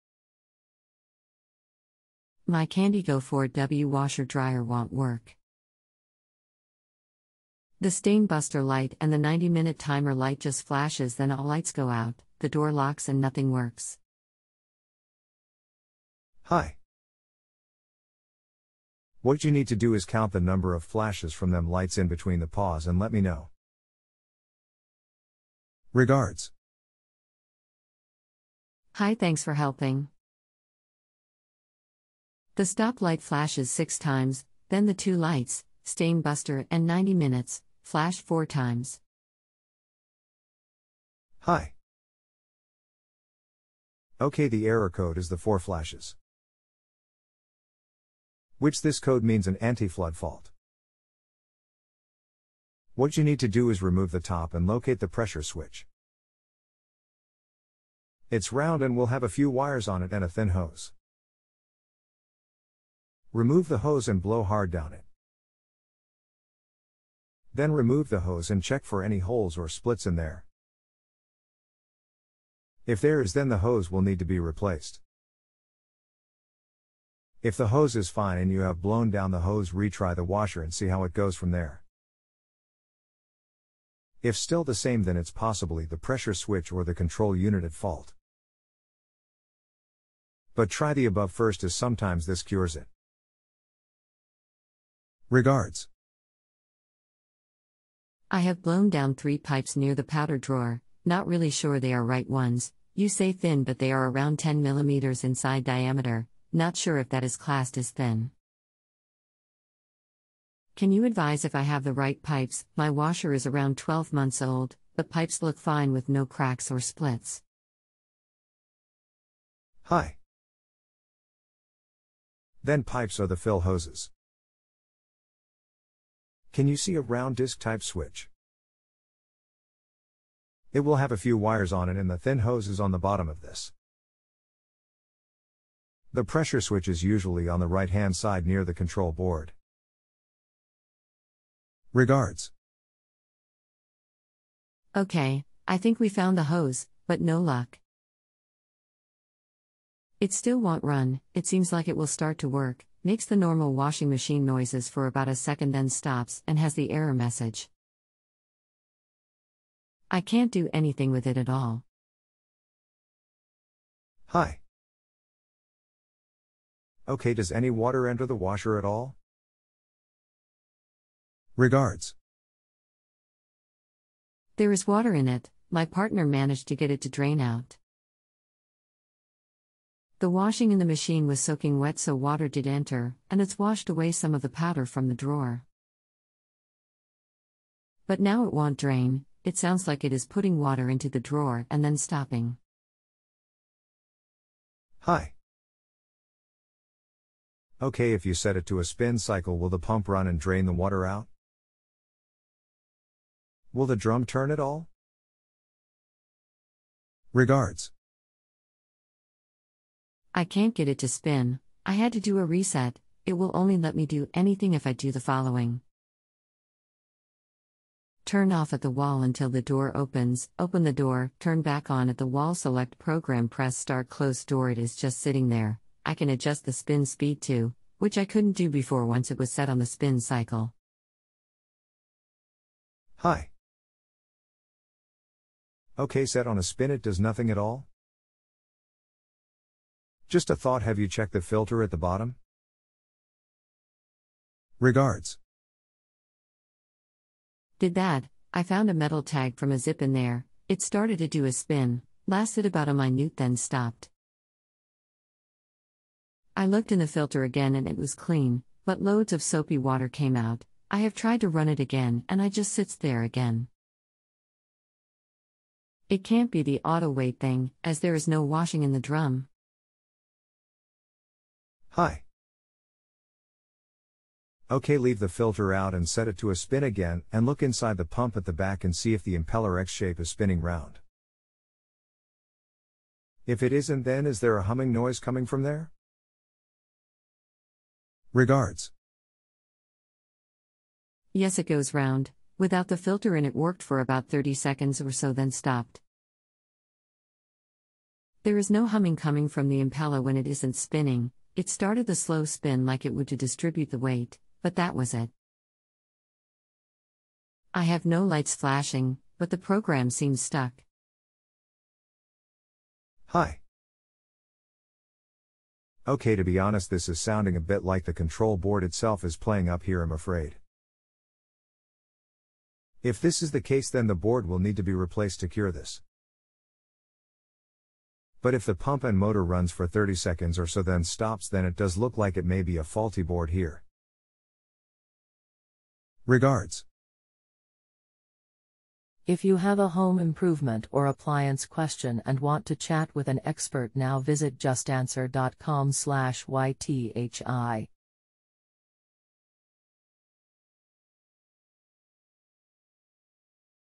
my candy go4w washer dryer wont work. the stain buster light
JustAnswer Customer: my candy go4w washer dryer wont work.
JustAnswer Customer: the stain buster light and the 90 min timer light just flashes then all lights go out, the door locks and nothing works
Daniel: Home Appliance Technician: Hi .
Daniel: Home Appliance Technician: What you need to do is count the number of flashes from them lights inbetween the pause and let me know.
Daniel: Home Appliance Technician: Regards
JustAnswer Customer: hi thanks for helping.
JustAnswer Customer: the stop light flashes 6 times, then the two lights (stain buster and 90 min) flashed 4 times
Daniel: Home Appliance Technician: Ok the error code is the 4 flashes.
Daniel: Home Appliance Technician: Which this code means an antiflood fault.
Daniel: Home Appliance Technician: What you need to do is remove the top and locate the pressure switch.
Daniel: Home Appliance Technician: Its round and will have a few wires on it and a thin hose.
Daniel: Home Appliance Technician: Remove the hose and blow hard down it.
Daniel: Home Appliance Technician: Then remove the hose and check for any holes or splits in there.
Daniel: Home Appliance Technician: If there is then the hose will need to be replaced.
Daniel: Home Appliance Technician: If the hose is fine and you have blown down the hose retry the washer and see how it goes from there.
Daniel: Home Appliance Technician: If still the same then its possibley the pressure switch or the control unit at fault.
Daniel: Home Appliance Technician: But try the above first as sometimes this cures it.
JustAnswer Customer: i have blown down 3 pipes near the powder drawer, not really sure they are right ones, you say thin but they are around 10mm inside diameter, not sure if that is classed as thin.
JustAnswer Customer: can you advise if i have the right pipes, my washer is around 12 months old, the pipes look fine with no cracks or splits
Daniel: Home Appliance Technician: Then pipes are the fill hoses.
Daniel: Home Appliance Technician: Can you see a round disc type switch.
Daniel: Home Appliance Technician: It will have a few wires on it and the thin hose is on the bottom of this.
Daniel: Home Appliance Technician: The pressure switch is usually on the right hand side near the control board.
JustAnswer Customer: ok, i think we found the hose, but no luck.
JustAnswer Customer: it still wont run, it seems like it will start to work, makes the normal washing machine noises for about a second then stops and has the error message.
JustAnswer Customer: i cant do anything with it at all,
Daniel: Home Appliance Technician: Ok does any water enter the washer at all?
JustAnswer Customer: there is water in it, my partner managed to get it to drain out.
JustAnswer Customer: the washing in the machine was soaking wet so water did enter, and its washed away some of the powder from the drawer.
JustAnswer Customer: but now it wont drain, it sounds like it is putting water into the drawer and then stopping
Daniel: Home Appliance Technician: Ok if you set it to a spin cycle will the pump run and drain the water out?
Daniel: Home Appliance Technician: Will the drum turn at all?
JustAnswer Customer: i cant get it to spin, i had to do a reset, it will only let me do anything if i do the following.
JustAnswer Customer: turn off at the wall untill the door opens, open the door, turn back on at the wall select program press start close door   it is just sitting there, i can adjust the spin speed too, which i couldnt do before once it was set on the spin cycle
Daniel: Home Appliance Technician: Ok set on a spin it does nothing at all?
Daniel: Home Appliance Technician: Just a thought have you checked the filter at the bottom?
JustAnswer Customer: did that, i found a metal tag from a zip in there, it started to do a spin, lasted about a minute then stopped.
JustAnswer Customer: i looked in the filter again and it was clean, but loads of soapy water came out,   i have tried to run it again and i just sits there again.
JustAnswer Customer: it cant be the auto weight thing, as there is no washing in the drum.
Daniel: Home Appliance Technician: Ok leave the filter out and set it to a spin again and look inside the pump at the back and see if the impeller X shape is spinning round.
Daniel: Home Appliance Technician: If it isnt then is there a humming noise comming from there?